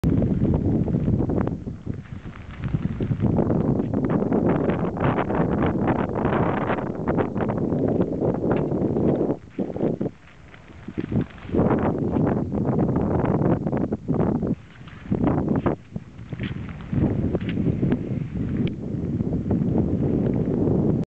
雪が降りましたIt snowed in Tokachi 2010/11/12
2010/11/12北海道十勝で雨まじりの雪が降りました。